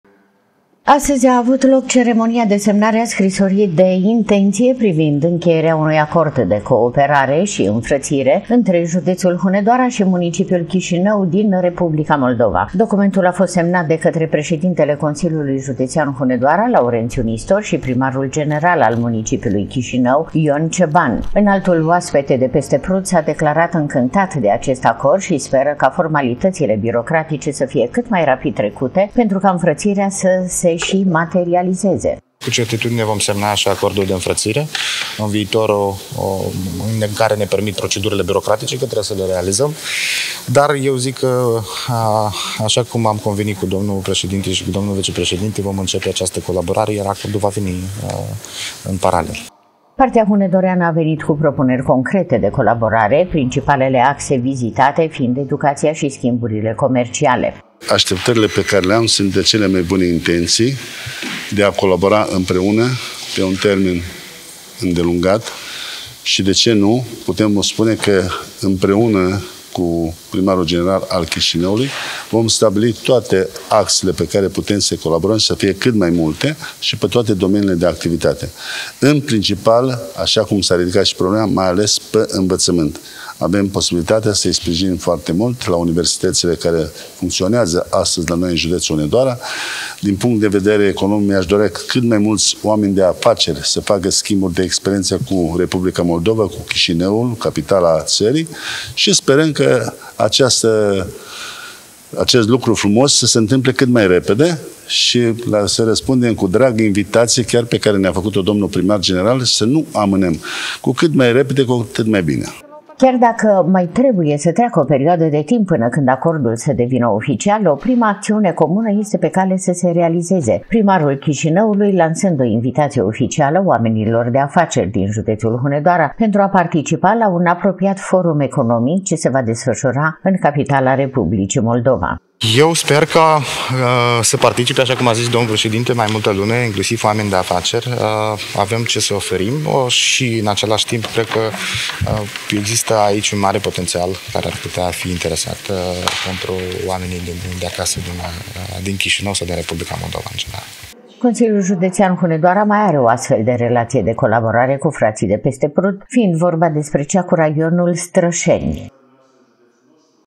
ÎNFRĂȚIRE ÎNTRE JUDEȚUL HUNEDOARA ȘI CHIȘINĂU
Județul Hunedoara are, de astăzi, o nouă relație de înfrățire. Este vorba despre o înfrățire cu o localitate care are o rezonanță aparte nu doar în sufletele hunedorenilor, ci ale tuturor românilor.